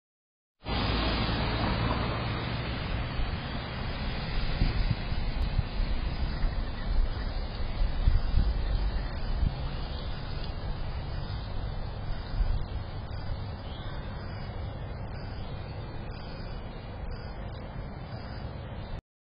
Amazon Vehicle Cut Through
Amazon vehicle cutting through stop sign February 2021